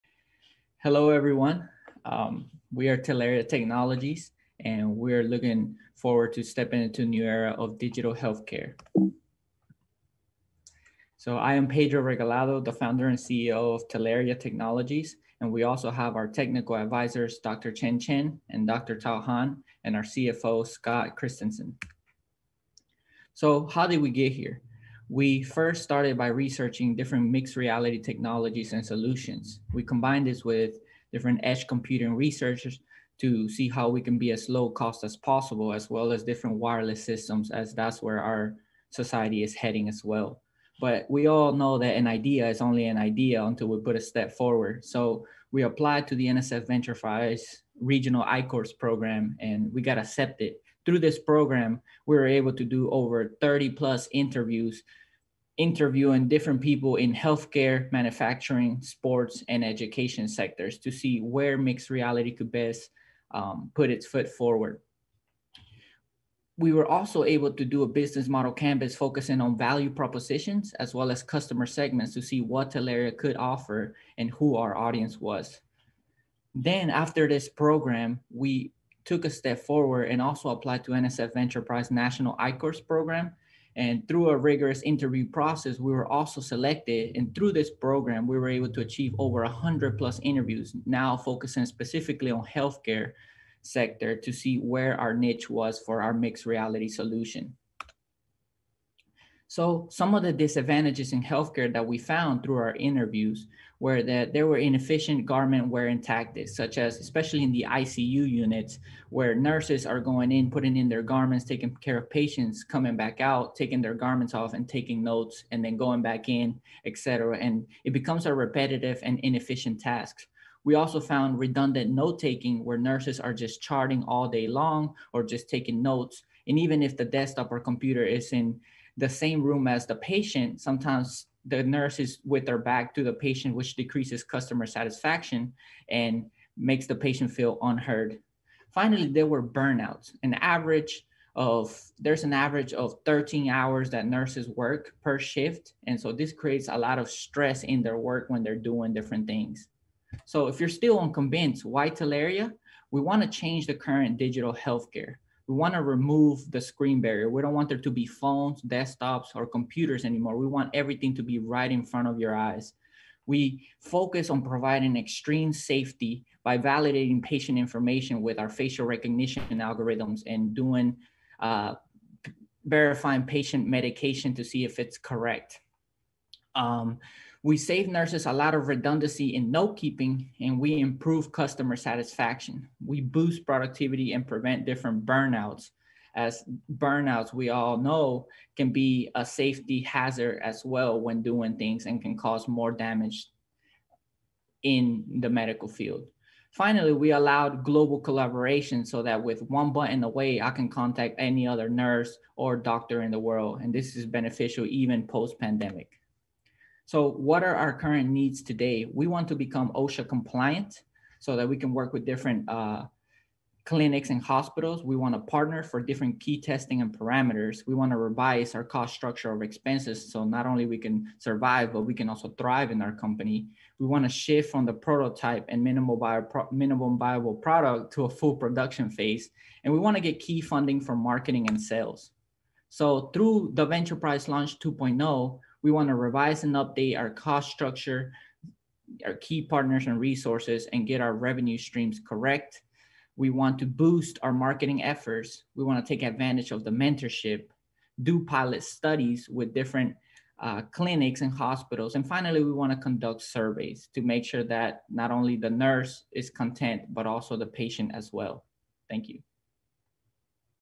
Pedro Vera Talaria Technologies
Talaria is a mixed reality telehealth system that aims to reduce at least 25% of the time nurses and doctors spend on filling out patient forms and charting so that they can spend more time helping the patient get the care that they need.